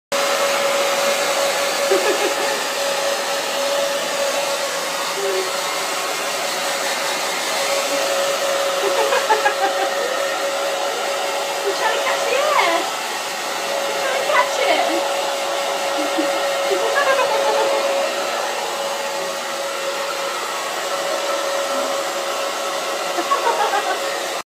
Howelsen 10 week Pyrenean Mountain Dog Puppy being groomed
This blow drying malarkey is super fun can't seem to catch the air though!